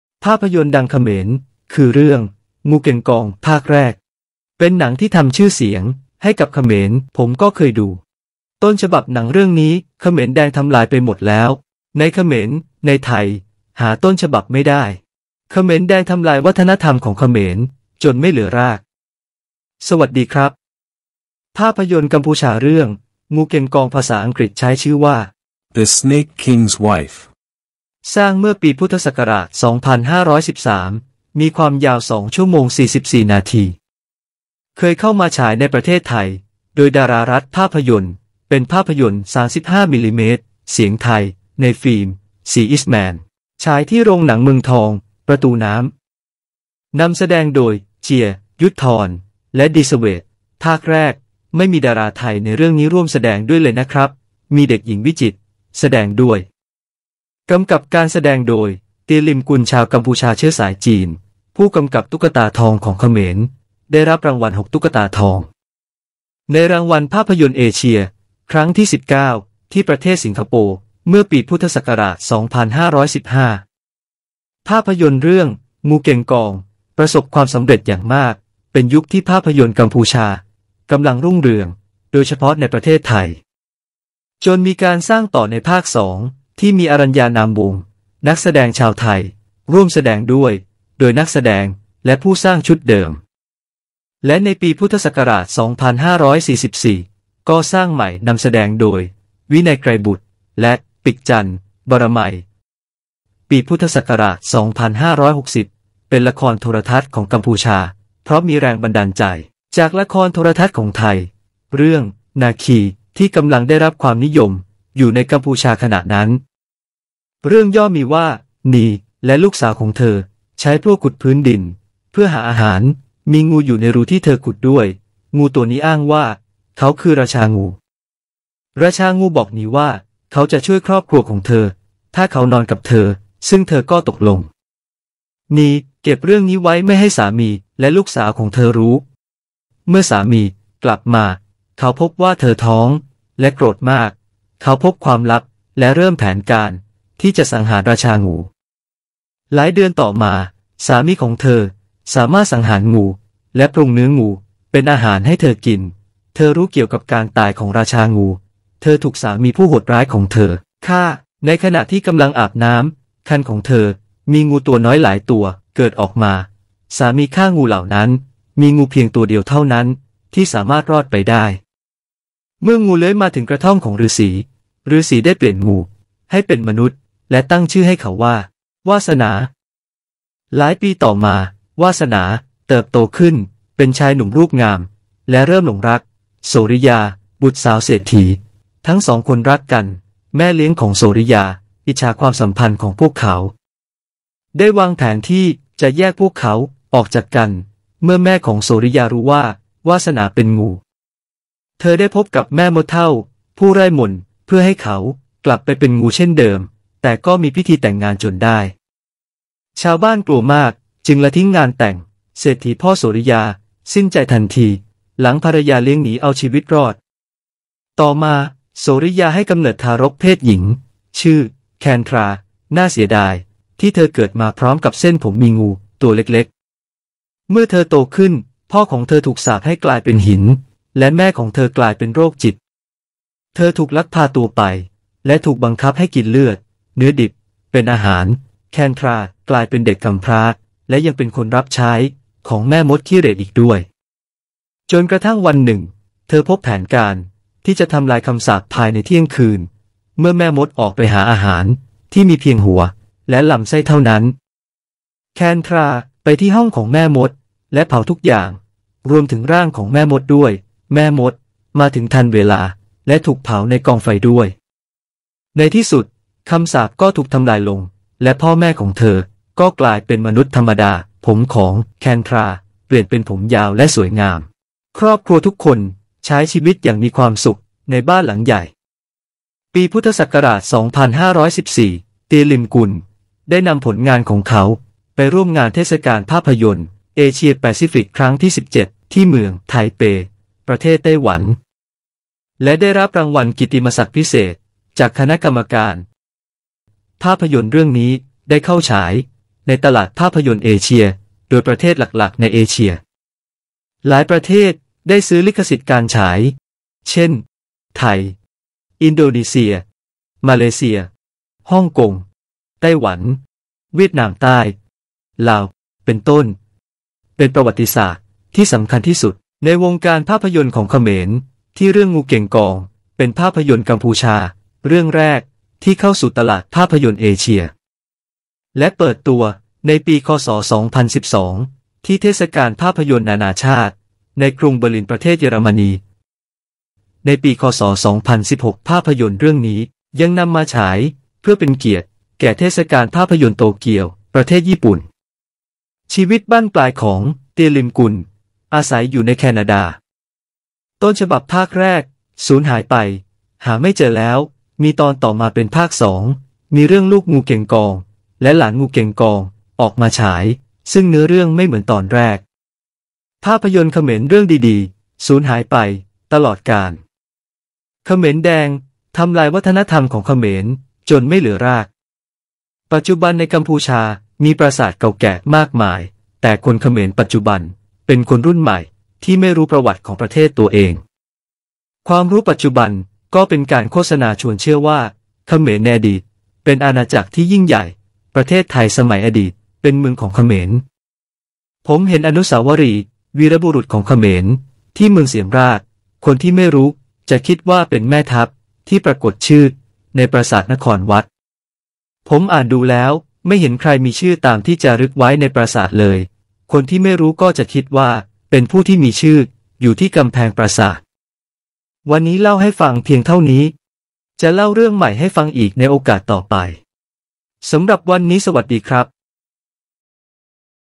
Ep71 ภาพยนตร์ดังของเขมร เรื่อง "งูเก็งกอง" กับต้นฉบับ ที่สูญหายไปตลอดกาล
ภาพยนตร์ดังเขมร คือเรื่อง งูเก็งกอง ภาคแรก เป็นหนังที่ทำชื่อเสียงให้กับเขมร ผมก็เคยดู ต้นฉบับหนังเรื่องนี้ เขมรแดงทำลายไปหมดแล้ว ในเขมร ในไทย หาต้นฉบับไม่ได้ เขมรแดงทำลายวัฒนธรรมของเขมร จนไม่เหลือราก
         ภาพยนตร์กัมพูชาเรื่อง งูเกงกอง (Pous Keng Kang)  (เขมร: ពស់កេងកង) ภาษาอังกฤษใช้ชื่อว่า The Snake King's Wife สร้างเมื่อปี พ.ศ. 2513 มีความยาว 2 ชม. 44 นาที เคยเข้ามาฉายในประเทศไทย โดยดารารัฐภาพยนตร์ เป็นภาพยนตร์ 35 ม.ม. เสียงไทยในฟิล์ม สีอิสต์แมน ฉายที่โรงหนังเมืองทอง ประตูน้ำ นำแสดงโดย เจีย ยุทธธร (លោក ជា យុទ្ធថន) และ ดี เสวต (លោកស្រី ឌី សាវ៉េត) ภาคแรกไม่มีดาราไทยในเรื่องนี้ร่วมแสดงด้วยเลยนะครับ มีเด็กหญิง วิจิตร แสดงด้วย กำกับการแสดงโดย เตียลิมกุน (ទា លឹមគុណ) ชาวกัมพูชาเชื้อสายจีน ผู้กำกับตุ๊กตาทองของเขมร ได้รับรางวัล 6 ตุ๊กตาทอง  ใน รางวัลภาพยนตร์เอเชีย (Asian Movie Awards) ครั้งที่ 19 ที่ประเทศสิงคโปร์เมื่อปี พ.ศ. 2515
         ภาพยนตร์เรื่อง งูเก็งกอง ประสพความสำเร็จอย่างมาก เป็นยุคที่ภาพยนตร์กัมพูชากำลังรุ่งเรือง โดยเฉพาะในประเทศไทย จนมีการสร้างต่อในภาค 2 ที่มี อรัญญา นามวงศ์ นักแสดงชาวไทยร่วมแสดงด้วย โดยนักแสดงและผู้สร้างชุดเดิม ดาราไทยประกอบด้วย สุคนธ์ คิ้วเหลี่ยม, ทองฮะ, ดาวน้อย ดวงใหญ่, ศศิสะบอง โดย
เตียลิมกุน  ผู้กำกับตุ๊กตาทอง  กำกับการแสดง ประจิต จุลพันธ์  สร้างบทพากย์ สมพงษ์ วงศ์รักไทย/จุรี โอศิริ พากย์ และในปี พ.ศ. 2544 ก็สร้างใหม่ นำแสดงโดย วินัย ไกรบุตร และปิค จัน บรมัย , ปี พ.ศ. 2560 เป็นละครโทรทัศน์ของกัมพูชา เพราะมีแรงบันดาลใจ จากละครโทรทัศน์ของไทยเรื่องนาคี ที่กำลังได้รับความนิยมอยู่ในกัมพูชาขณะนั้น
          เรื่องย่อมีว่า นี และลูกสาวของเธอใช้พลั่วขุดพื้นดินเพื่อหาอาหาร  มีงูอยู่ในรูที่เธอขุดด้วย งูตัวนี้อ้างว่าเขาคือราชางู  ราชางูบอกนีว่า เขาจะช่วยครอบครัวของเธอถ้าเขานอนกับเธอ ซึ่งเธอก็ตกลง นี เก็บ เรื่องนี้ไว้ไม่ให้สามีและลูกสาวของเธอรู้ เมื่อสามี กลับมา เขาพบว่าเธอท้องและโกรธมาก เขาพบความลับและเริ่มแผนการที่จะสังหารราชางู หลายเดือนต่อมา สามีของเธอ สามารถสับงูและปรุงเนื้องูเป็นอาหารให้เธอกิน เธอรู้เกี่ยวกับการตายของราชางู เธอถูกสามีผู้โหดร้ายของเธอฆ่า ในขณะที่กำลังอาบน้ำ ครรภ์ของเธอ มีงูตัวน้อยหลายตัวเกิดออกมา สามีฆ่างูเหล่านั้น มีงูเพียงตัวเดียวเท่านั้นที่สามารถอยู่รอดได้ เมื่อมาถึงกระท่อมของฤาษี ฤาษีได้แปลงร่างเขาให้เป็นมนุษย์ และตั้งชื่อให้เขาว่า วาสนา
          หลายปีต่อมา วาสนา เติบโตขึ้น เป็นชายหนุ่มรูปงามและเริ่มหลงรัก โสริยา บุตรสาวเศรษฐี ทั้งสองคนรักกัน แม่เลี้ยงของโสริยาที่อิจฉาความสัมพันธ์ของพวกเขา ได้วางแผนที่จะแยกพวกเขาออกจากกัน เมื่อแม่ของโสริยารู้ว่า วาสนาเป็นงู เธอได้พบกับแม่มดเฒ่าผู้ร่ายมนตร์เพื่อให้ขากลับไปเป็นงูเช่นเดิม แต่ก็มีพิธีแต่งงานจนได้ เกิดเหตุการณ์ในงาน ชาวบ้านกลัวมากจึงละทิ้งงานแต่ง เศรษฐีพ่อโสริยาสิ้นใจทันทีหลังภรรยาเลี้ยงหนีเอาชีวิตรอด
          ต่อมา โสริยาให้กำเนิดทารกเพศหญิงชื่อ แคนทรา น่าเสียดายที่เธอเกิดมาพร้อมกับเส้นผมมีงู งูตัวเล็ก ๆ
เมื่อเธอโตขึ้น พ่อของเธอถูกสาปให้กลายเป็นหิน และแม่ของเธอกลายเป็นโรคจิต เธอถูกลักพาตัวไปและถูกบังคับให้กินเลือดเนื้อดิบเป็นอาหาร แคนทรา กลายเป็นเด็กกำพร้าและยังเป็นคนรับใช้ของแม่มดขี้เหร่อีกด้วย
จนกระทั่งวันหนึ่งเธอพบแผนการที่จะทำลายคำสาปภายในเที่ยงคืน เมื่อแม่มดออกหาอาหารที่มีเพียงหัวและลำไส้เท่านั้น แคนทราไปที่ห้องล็อกเกอร์ของแม่มดและเผาทุกอย่างรวมถึงร่างของแม่มดด้วย แม่มดมาถึงทันเวลาและถูกเผาในกองไฟด้วย
        ในที่สุด คำสาปก็ถูกทำลายลง และพ่อแม่ของเธอก็กลายเป็นมนุษย์ธรรมดา ผมของ แคนทรา เปลี่ยนเป็นผมยาวและสวยงาม ครอบครัวทุกคนใช้ชีวิตอย่างมีความสุขในบ้านหลังใหญ่
         ปี พ.ศ. 2514 Tea Limkun ได้นำผลงานของเขาไปร่วมงานเทศกาลภาพยนตร์เอเชียแปซิฟิกครั้งที่ 17 ที่เมืองไทเป ประเทศไต้หวัน และได้รับรางวัลกิตติมศักดิ์พิเศษจากคณะกรรมการ ภาพยนตร์เรื่องนี้ได้เข้าฉายในตลาดภาพยนตร์เอเชีย โดยประเทศหลักๆ ในเอเชีย หลายประเทศได้ซื้อลิขสิทธิ์การฉาย เช่น ไทย อินโดนีเซีย มาเลเซีย ฮ่องกง ไต้หวัน เวียดนามใต้ ลาว เป็นต้น เป็นประวัติศาสตร์ที่สำคัญที่สุด ในวงการภาพยนตร์ของเขมรที่เรื่อง งูเก็งกอง เป็นภาพยนตร์กัมพูชาเรื่องแรกที่เข้าสู่ตลาดภาพยนตร์เอเชียก่อน และเปิดตัวในปี ค.ศ. 2012 ที่เทศกาลภาพยนตร์นานาชาติในกรุงเบอร์ลิน ประเทศเยอรมนี ในปี ค.ศ. 2016 ภาพยนตร์เรื่องนี้ ยังนำมาฉายเพื่อเป็นเกียรติแก่เทศกาลภาพยนตร์โตเกียว ประเทศญี่ปุ่น
          ชีวิตบั้นปลายของ เตีย ลิม กุน  (Tea Lim Kun) อาศัยอยู่ในแคนาดา ต้นฉบับภาคแรก สูญหายไปหาไม่เจอแล้ว มีตอนต่อมาเป็นภาค 2 มีเรื่องลูกงูเก็งกอง และหลานงูเก็งกองออกมาฉาย ซึ่งเนื้อเรื่องไม่เหมือนตอนแรก ภาพยนตร์เขมรเรื่องดีๆ สูญหายไปตลอดกาล เขมรแดงทำลายวัฒนธรรมของเขมร จนไม่เหลือราก
ปัจจุบันในกัมพูชามีปราสาทเก่าแก่มากมาย แต่คนเขมรปัจจุบันเป็นคนรุ่นใหม่ที่ไม่รู้ประวัติของประเทศตัวเอง
ความรู้ปัจจุบันก็เป็นการโฆษณาชวนเชื่อว่า เขมรในอดีตเป็นอาณาจักรที่ยิ่งใหญ่ ประเทศไทยสมัยอดีตเป็นเมืองของเขมร
ผมเห็นอนุสาวรีย์วีรบุรุษของเขมรที่เมืองเสียมราฐ คนที่ไม่รู้ จะคิดว่าเป็นแม่ทัพที่ปรากฏชื่อในปราสาทนครวัด ผมอ่านดูแล้วไม่เห็นใครมีชื่อตามที่จารึกไว้ในปราสาทเลย คนที่ไม่รู้ก็จะคิดว่าเป็นผู้ที่มีชื่ออยู่ที่กำแพงปราสาท.